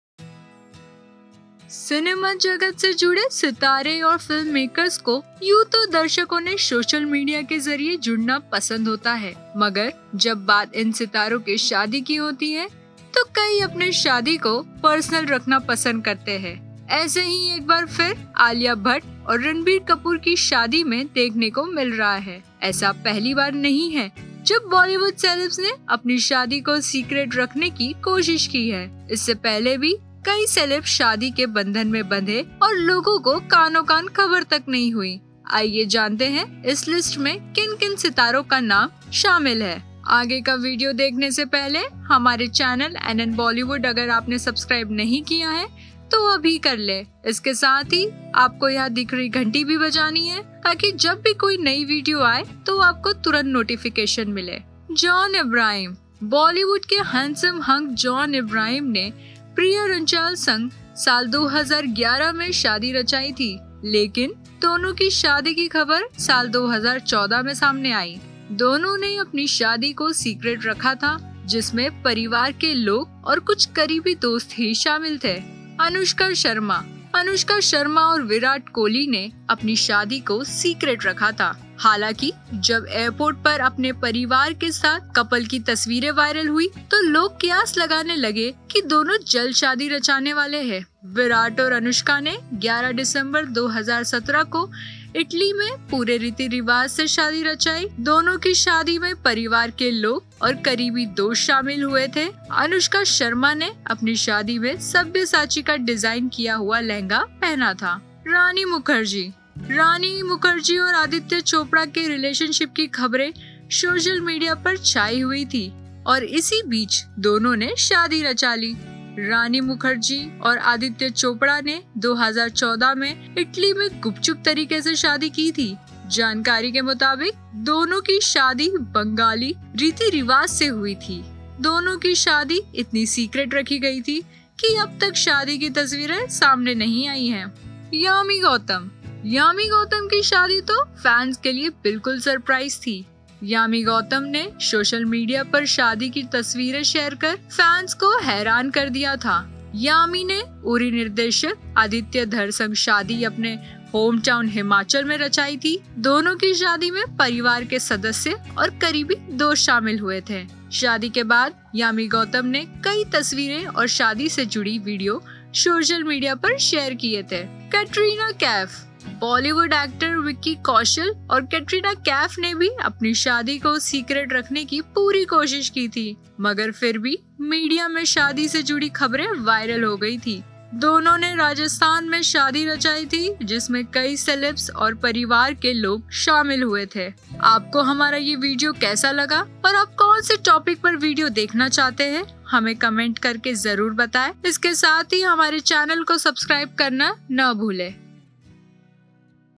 Ranbir-Alia से पहले इन सेलेब्स ने सीक्रेट रखी शादी | NN Bollywood
सिनेमा जगत से जुड़े सितारों और फिल्ममेकर्स को यूं तो दर्शकों से सोशल मीडिया के जरिए जुड़ना पसंद होता है मगर जब बात इन सितारों की शादी की होती है तो कई अपनी शादी को पर्सनल रखना पसंद करते हैं. ऐसा ही एक बार फिर आलिया भट्ट और रणबीर कपूर की शादी में देखने को मिल रहा है. ऐसा पहली बार नहीं है जब बॉलीवुड सेलेब्स ने अपनी शादी को सीक्रेट रखने की कोशिश की है इससे पहले भी कई सेलेब्स शादी के बंधन में बंधे हैं और लोगों को कानोंकान खबर तक नहीं हुई. आइए जानते हैं इस लिस्ट में किन-किन सितारों का नाम है शामिल.